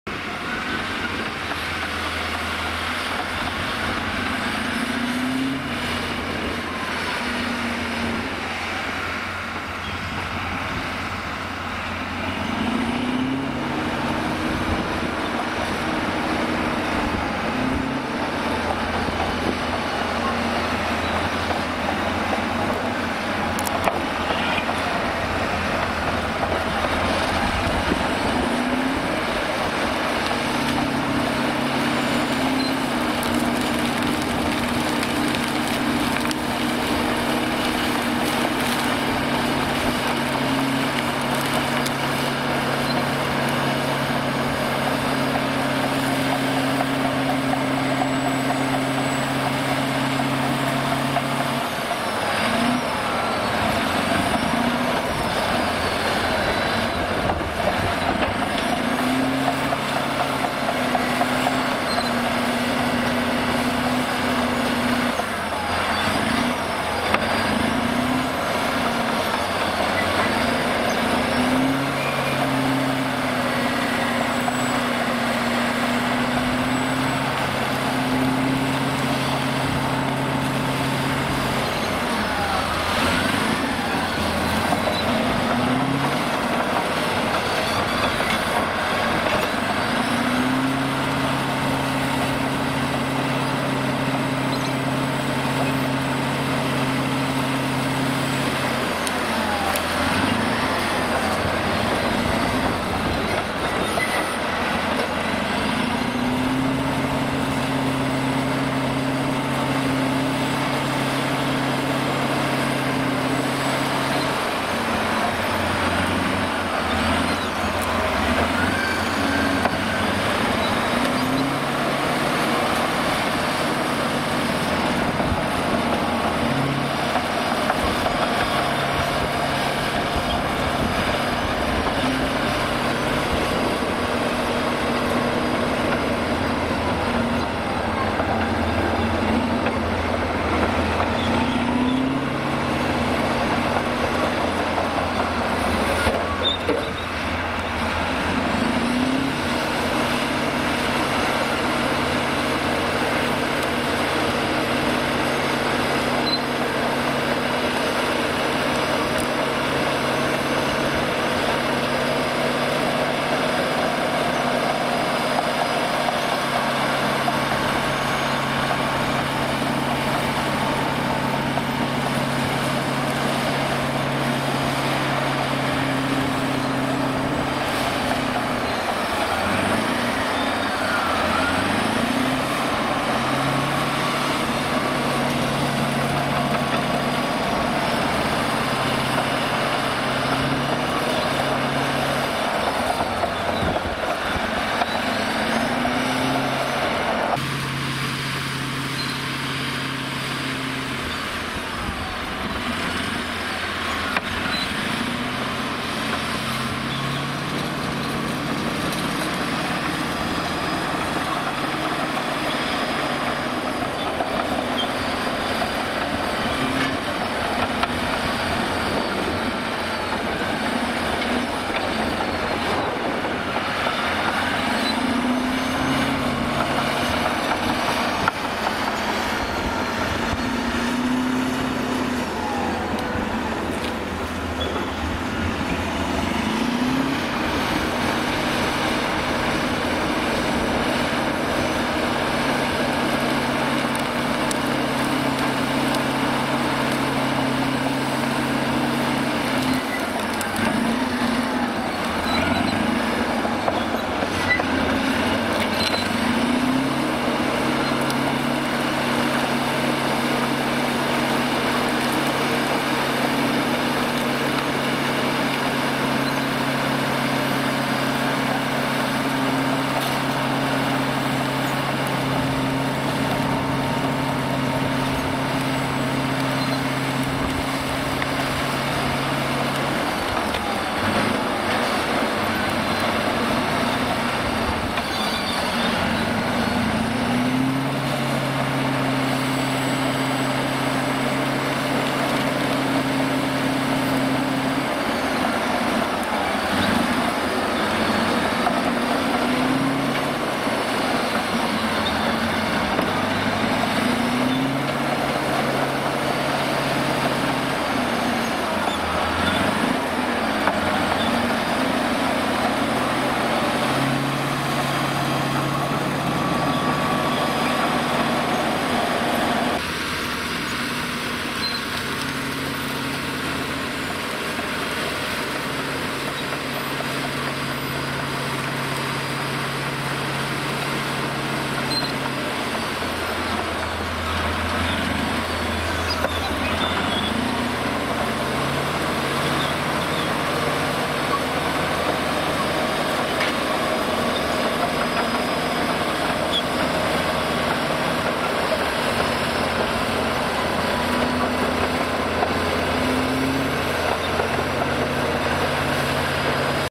Бульдозер Cat D9R и экскаватор JCB JS305LC корчуют пни часть 2
Бульдозер Cat D9R и экскаватор JCB JS305LC корчуют и перемещают пни и порубочные остатки на строительстве трассы М12
Cat D9R dozer and JCB JS305LC crawler excavator pulling and moving stumps on a highway construction project